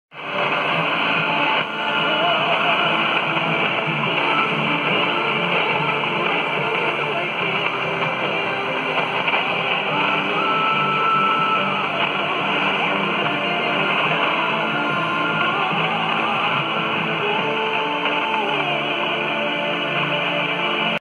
Radio Relogio 4905KHz Rio de Janeiro, Brasil - 17FEB2017 04:21 utc
Radio Relogio of Rio de Janeiro is back after some weeks off the air. Wrongfully listed as Radio Anhanguera. Tuned in Terra de Areia-RS, southern Brasil.